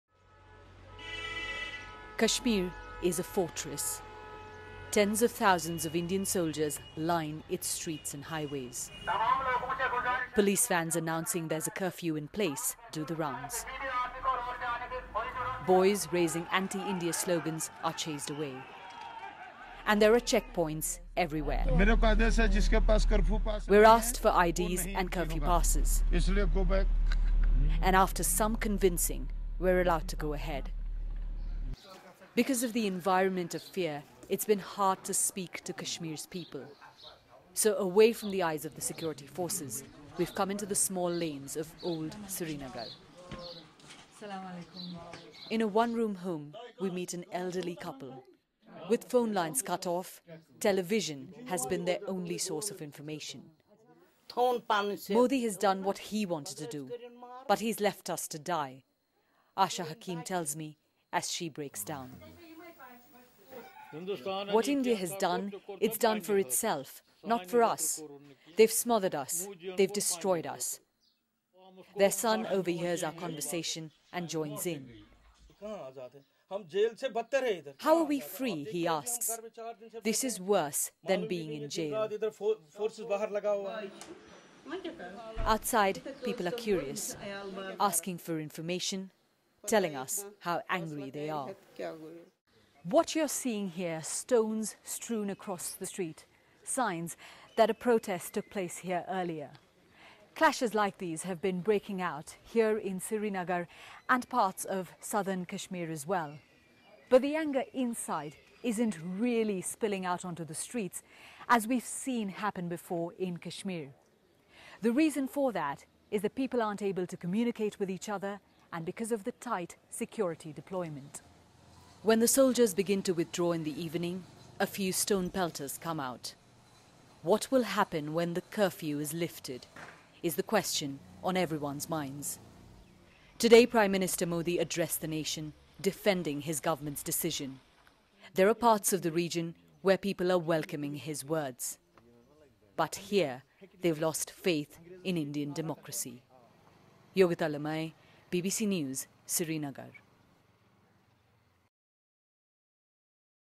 Kashmir: 'Modi has left us to die' - BBCURDU
Article 370: How people of Srinagar are living in curfew like situation?